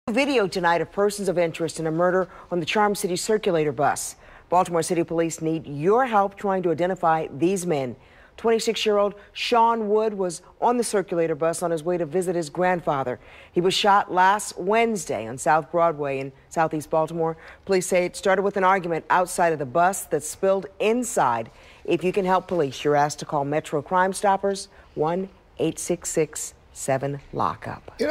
BPD releases video of bus shooting suspects
Baltimore police need your help identifying people in surveillance video connected to a fatal shooting on a Charm City Circulator bus.